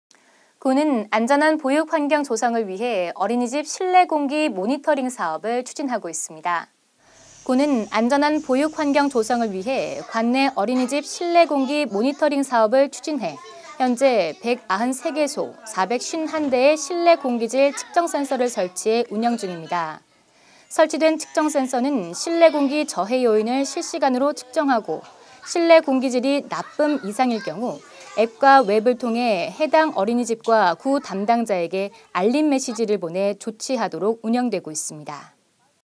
어린이집 실내공기질 모니터링 사업
구는 안전한 보육환경 조성을 위해 어린이집 실내공기 모니터링 사업을 추진하고 있습니다.

구는 안전한 보육환경 조성을 위해 관내 어린이집 실내공기 모니터링 사업을 추진해 현재 193개소 451대의 실내 공기질 측정센서를 설치해 운영 중입니다.

설치된 측정센서는 실내공기 저해요인을 실시간으로 측정하고 실내 공기질이 ‘나쁨’ 이상일 경우, 앱과 웹을 통해 해당 어린이집과 구 담당자에게 ‘알림메세지’을 보내 조치하도록 운영되고 있습니다.